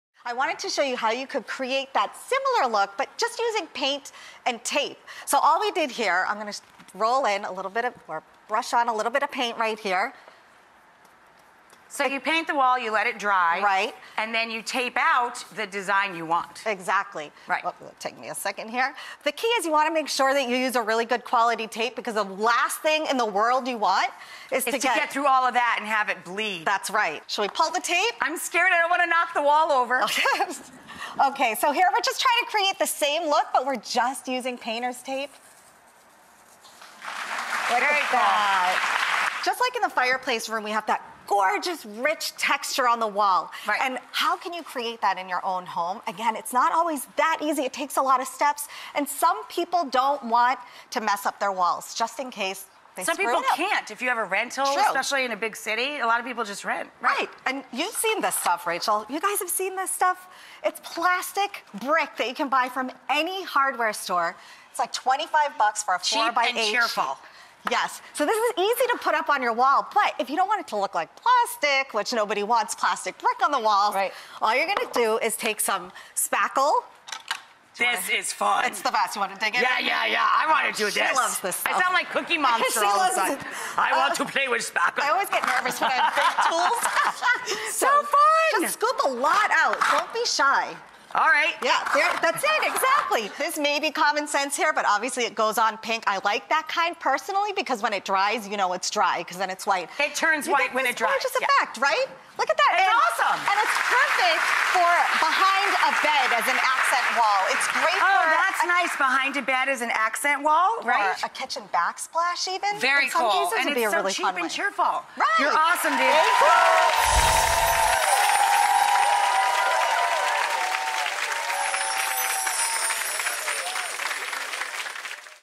How to Add Texture to a Wall With Tape + Plastic Brick (?!)
Design pro Taniya Nayak shows us how it's done!